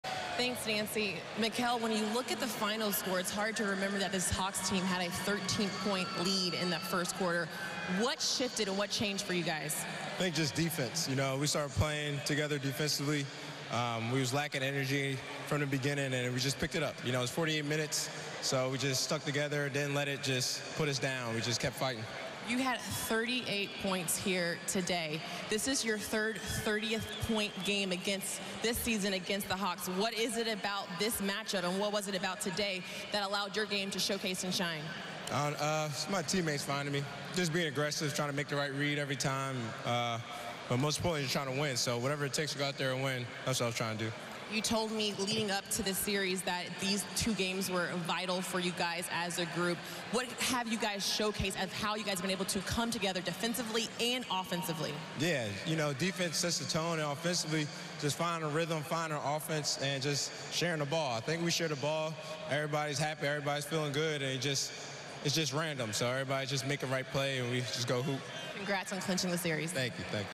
Mikal Bridges drops 38 points in win over Hawks
Mikal Bridges says defense led to offense as the Nets pull off a comeback victory over the Hawks in Brooklyn.